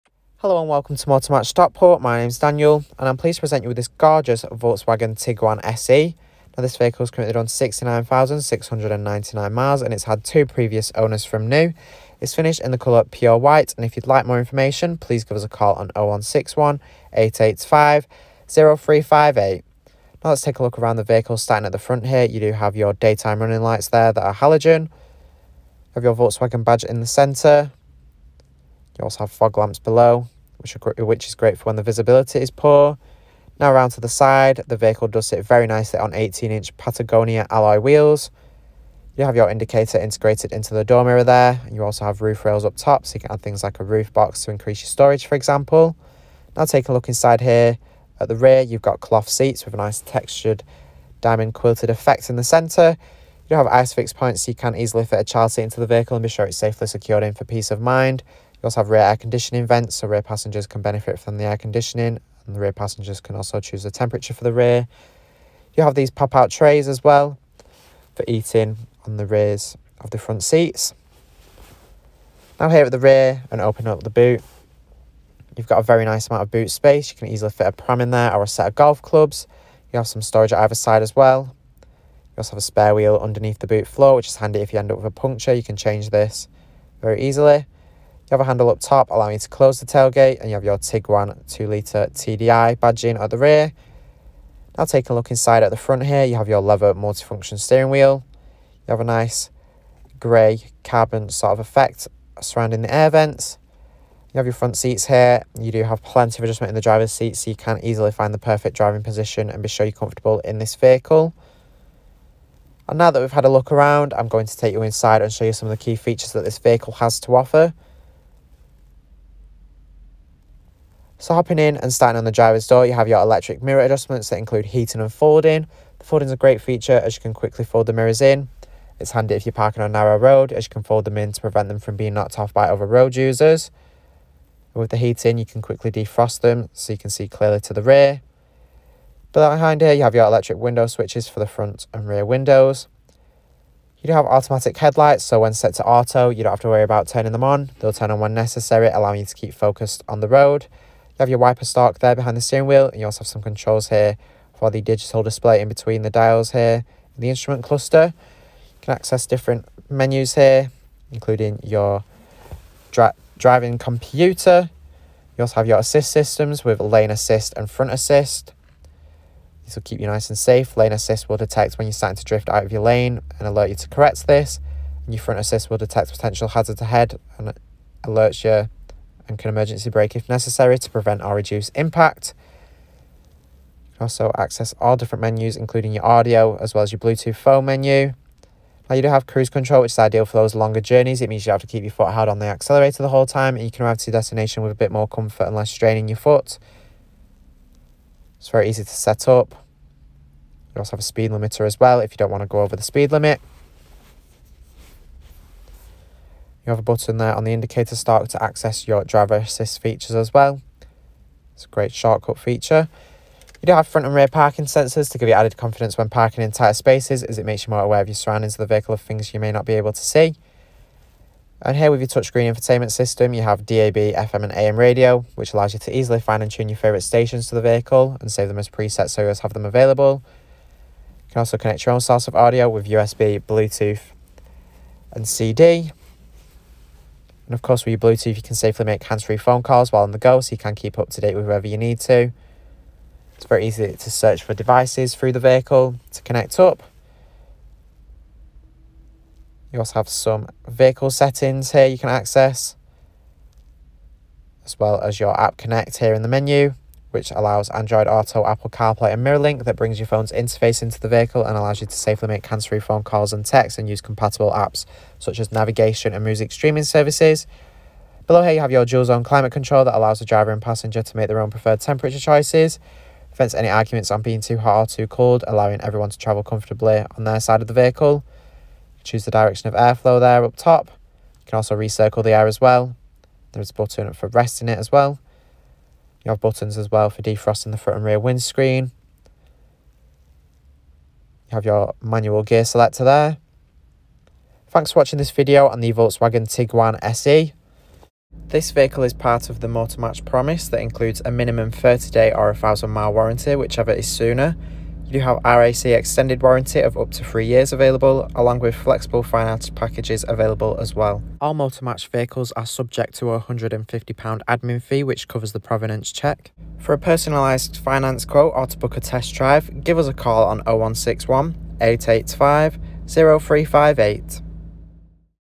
Approved Used Volkswagen Tiguan SE TDI | Motor Match Stockport
Just arrived in stock this Volkswagen Tiguan SE is now available for sale from Motor Match Stockport.

Reg - GM17 LAA
Mileage - 69,699
Transmission - Manual
Fuel - Diesel

Key Features;
Parking sensors
Cruise control
Front assist and Lane keeping assist
Apple CarPlay/Android Auto/MirrorLink